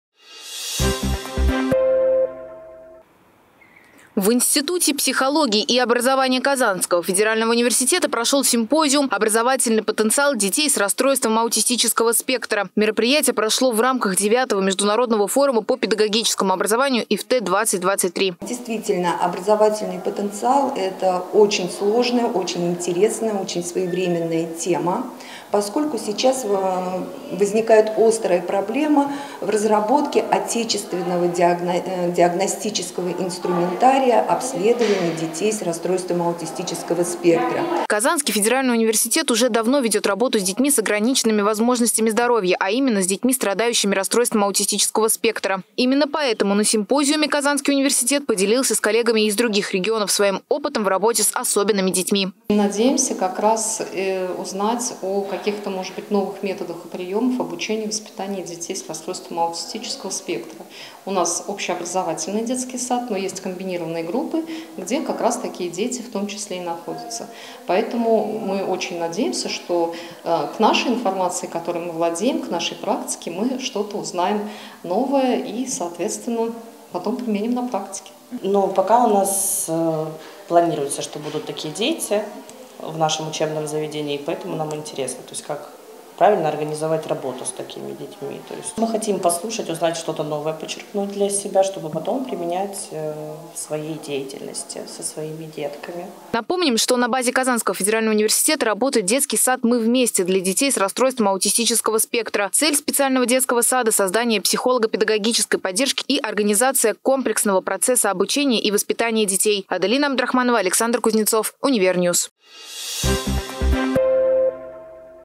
КАК РАЗВИВАТЬ ДЕТЕЙ В СОВРЕМЕННОМ МИРЕ
В Институте психологии и образования Казанского федерального университета прошел симпозиум, посвященный образовательному потенциалу детей с расстройствами аутистического спектра. Подробнее расскажет моя коллега Аделина Абдрахманова.